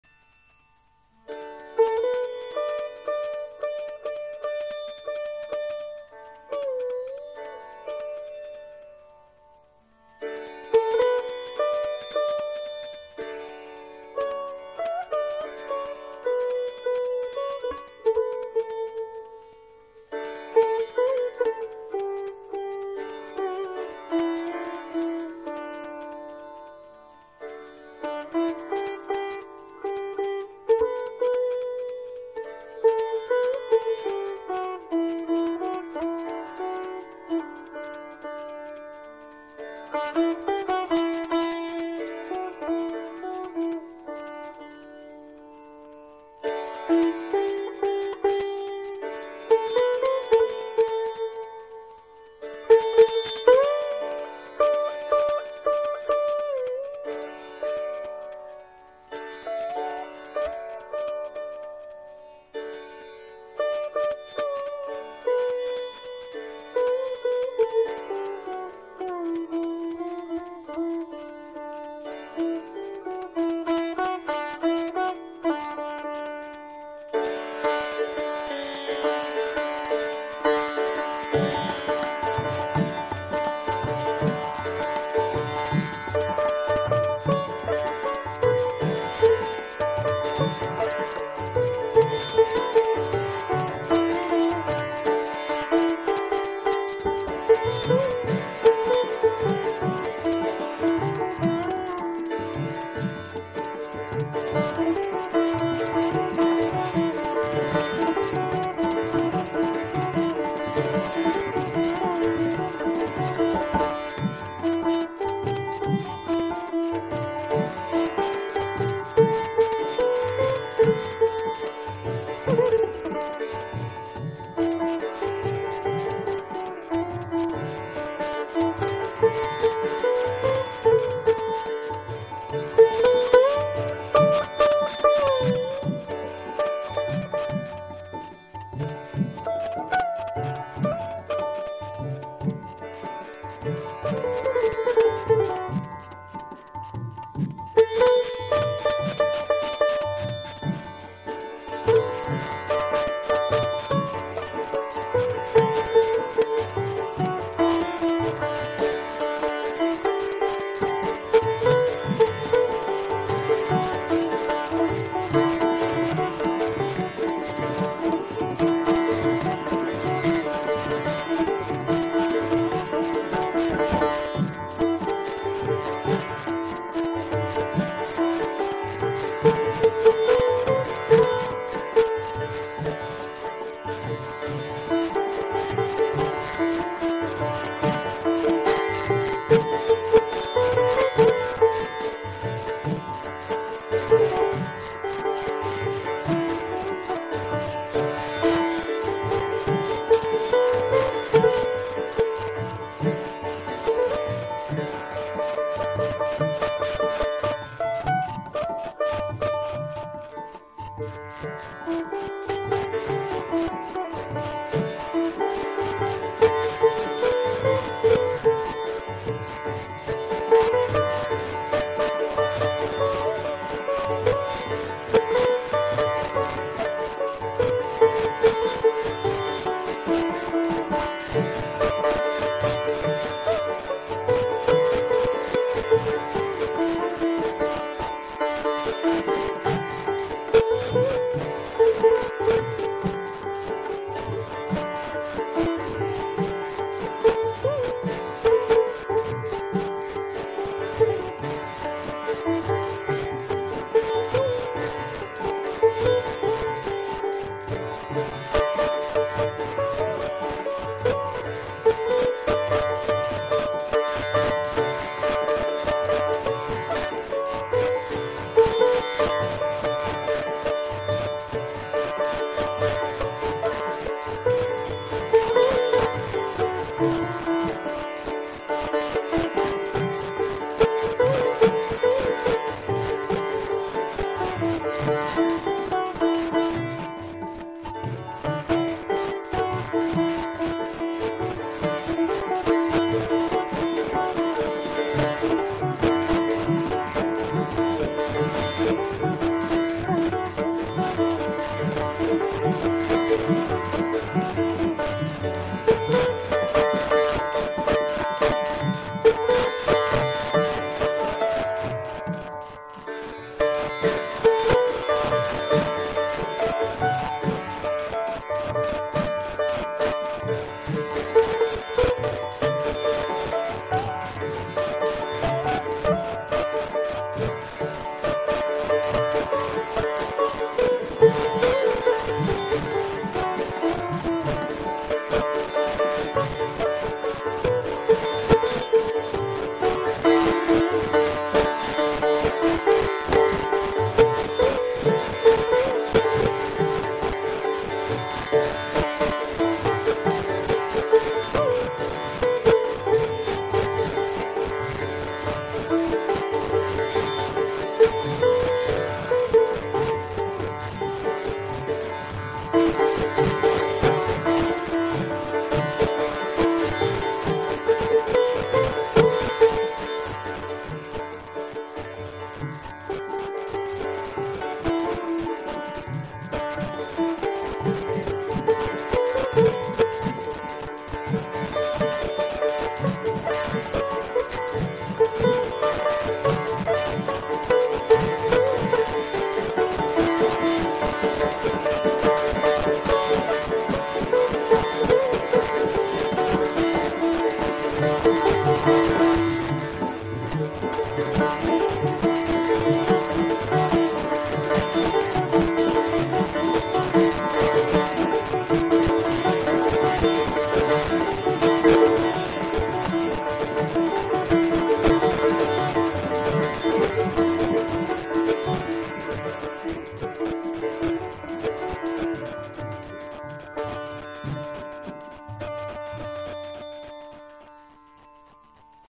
Kalyani Roy- Sitar-BHATIYALI SONG
BHATIYALI SONG
Surasree Kalyani Roy- Sitar
Tabla : Samir Chatterjee
Tanpura : Debangshu Mukherjee

NOTE : EXTREMELY SORRY FOR SCRATCH SOUND

Help Please.